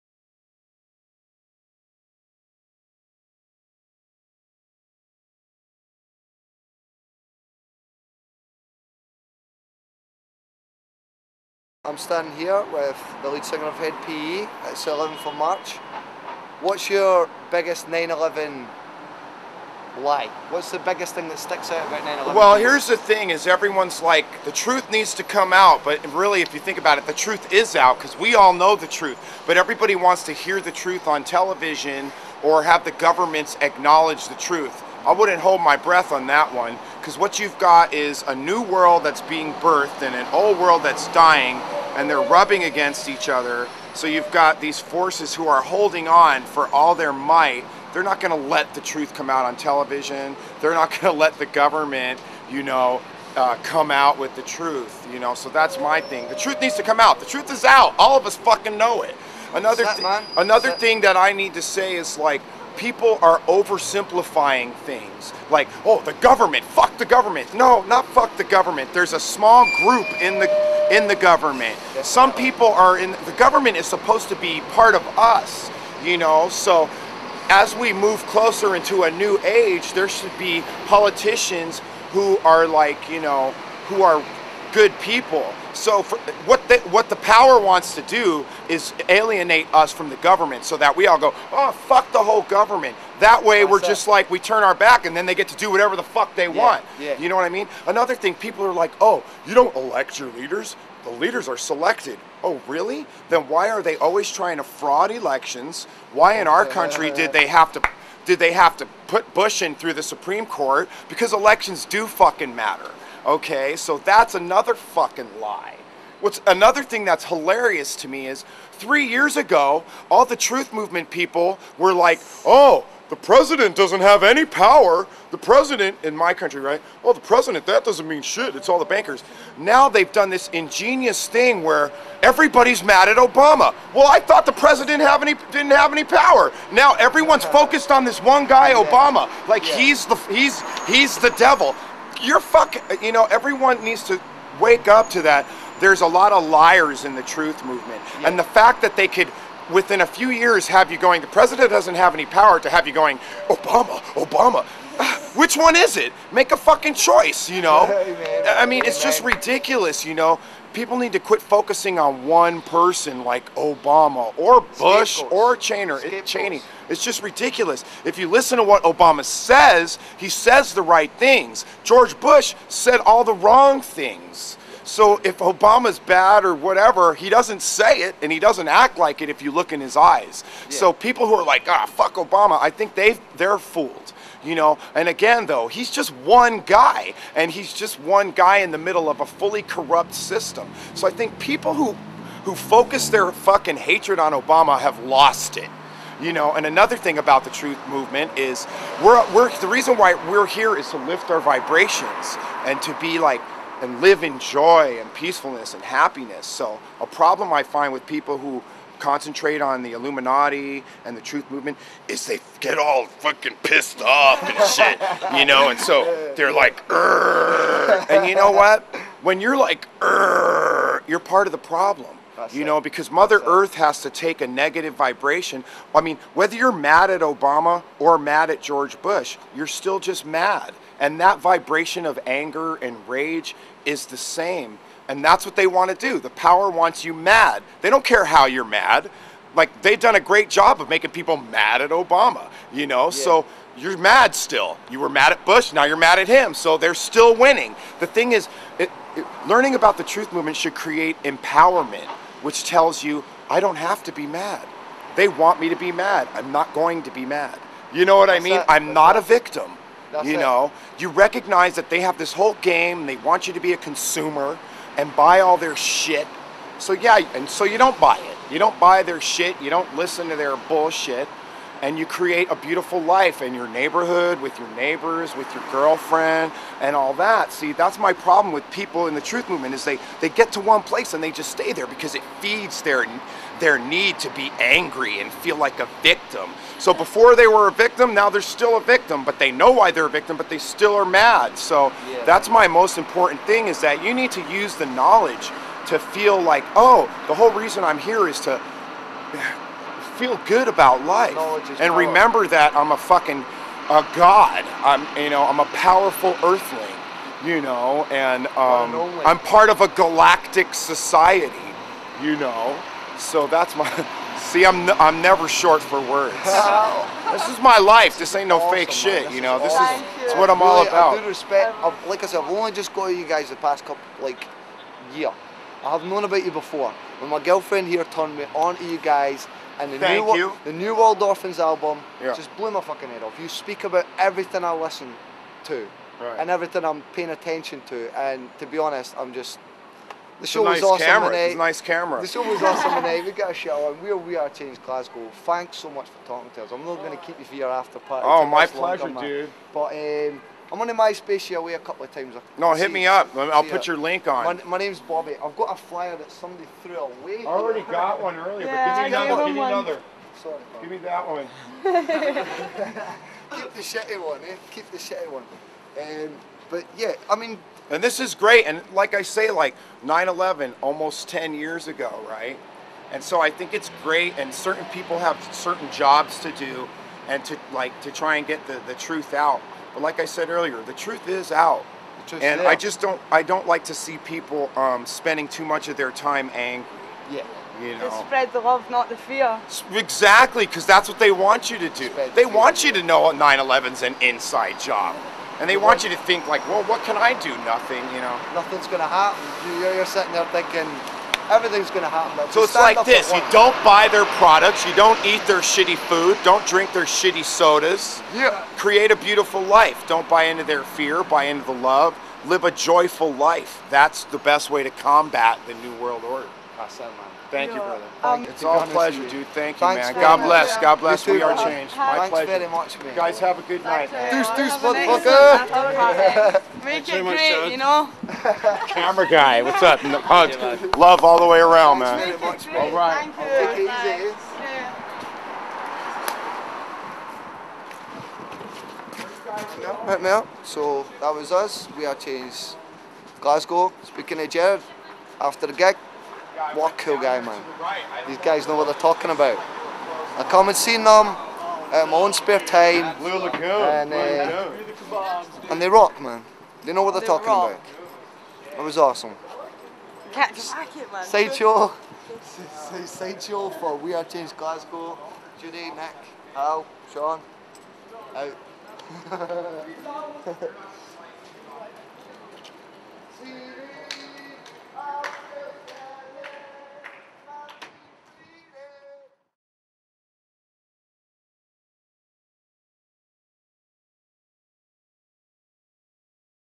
We Are Change Glasgow - Hed PE - Jared Gomes
A post-gig interview with Jared Gomes, lead vocalist of Hed PE, on the subject of the truth movement.